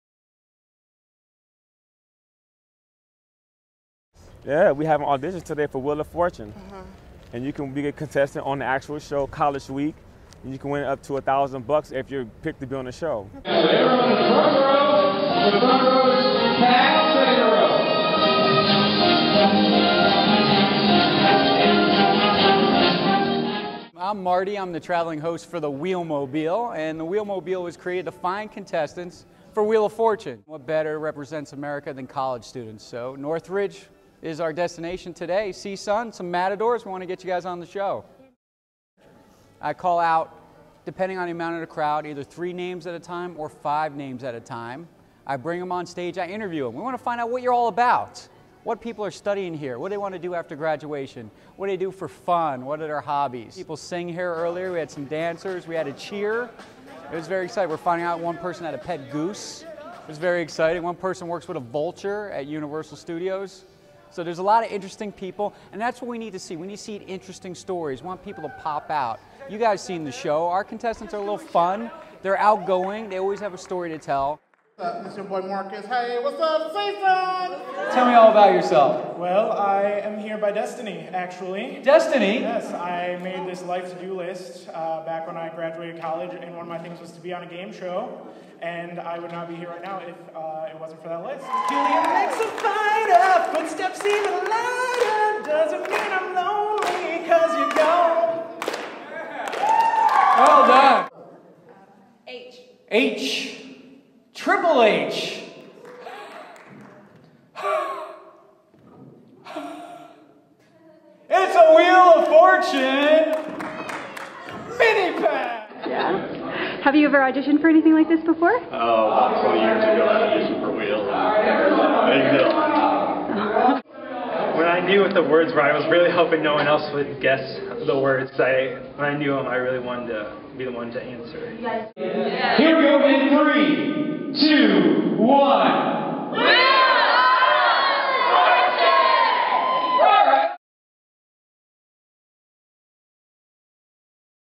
Wheel of Fortune holds auditions at CSUN
Wheel of Fortune holds auditions at CSUN for the college edition.